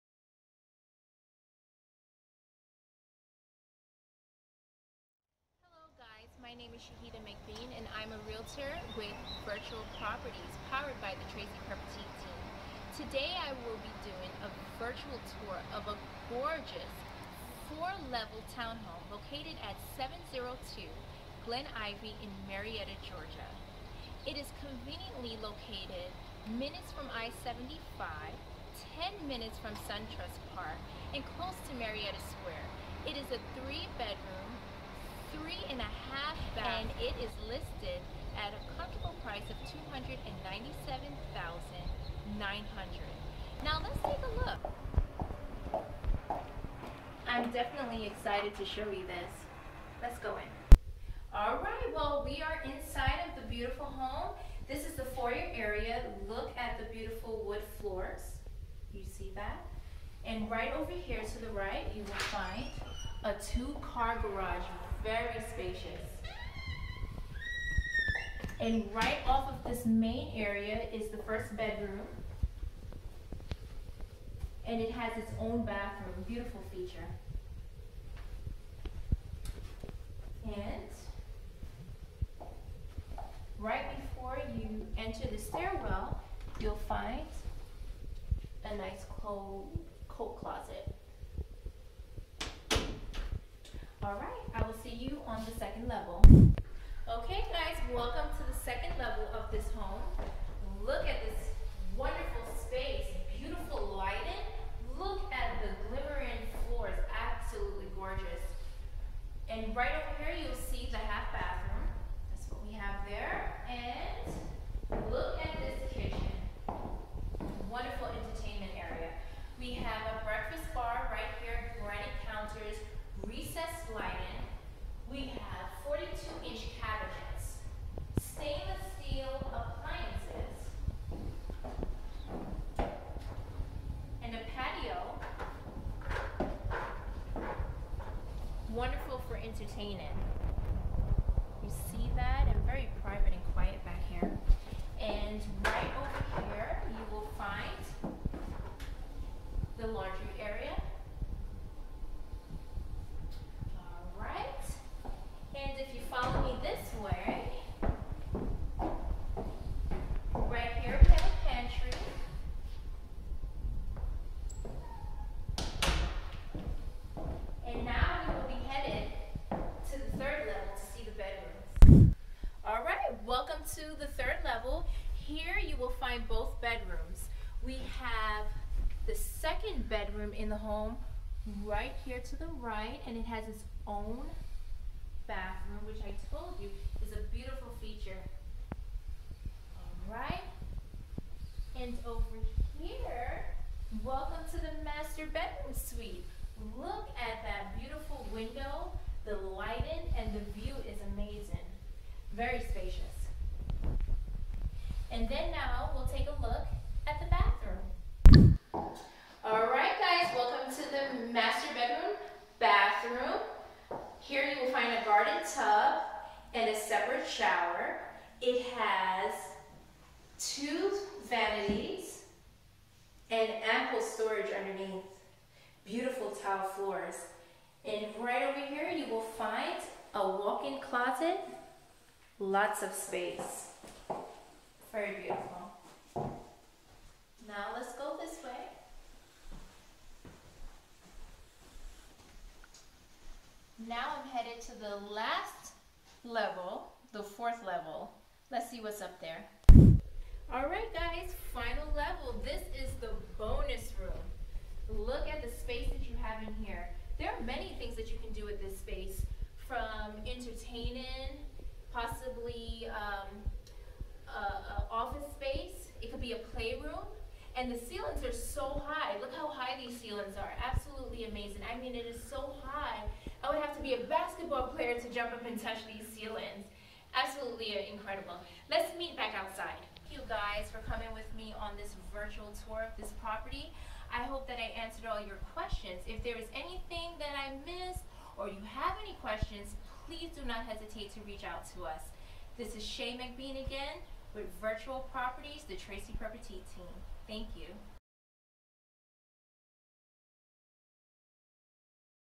Great Location in Cobb County!! 702 Glen Ivy Marietta GA - Real Estate Virtual Tours/ Open Houses
Amazing 4 level Townhome in Gated community minutes to I-75, the Marietta Square and new Marietta Market, Kennesaw Mountain, KSU and more!. Exquisite trim package, glimmering hardwoods, New Carpet and freshly painted. Very open floor plan. Kitchen with breakfast bar open to dining room and great room. Fireplace w/gas starter. Gorgeous 42" cabinets in kitchen, gas range, granite counters & tile backsplash and recessed lighting. Spacious master bedroom has bath with double sink vanities, tile floor, separate garden tub and shower. Walk in on main first floor to finished bedroom, with private full bath. Each bedroom has its own private full bathroom. Large 4th floor is an amazing bonus room/bedroom/office/playroom/exercise room ... whatever you need it to be! Through the French doors leads you to the private back deck and yard. The neighborhood amenities include Pool and Tennis and nearby shopping and dining are excellent!